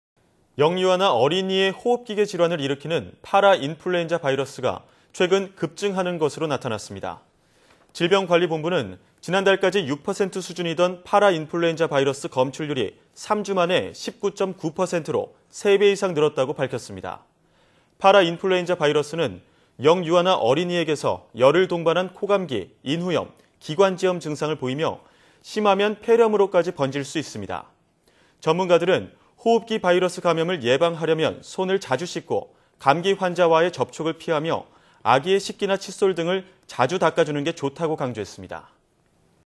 호흡기계 질환 '파라인플루엔자 바이러스' 급증 / YTN 사이언스
영유아나 어린이의 호흡기계 질환을 일으키는 파라인플루엔자 바이러스가 최근 급증하는 것으로 나타났습니다.

질병관리본부는 지난달까지 6% 수준이던 파라인플루엔자 바이러스 검출률이 3주 만에 19.9%로 3배 이상 늘었다고 밝혔습니다.

파라인플루엔자 바이러스는 영유아나 어린이에게서 열을 동반한 코감기, 인후염, 기관지염 증상을 보이며 심하면 폐렴으로까지 번질 수 있습니다.

전문가들은 호흡기 바이러스 감염을 예방하려면 손을 자주 씻고 감기 환자와의 접촉을 피하며 아기의 식기나 칫솔 등은 자주 닦아주는 게 좋다고 강조했습니다.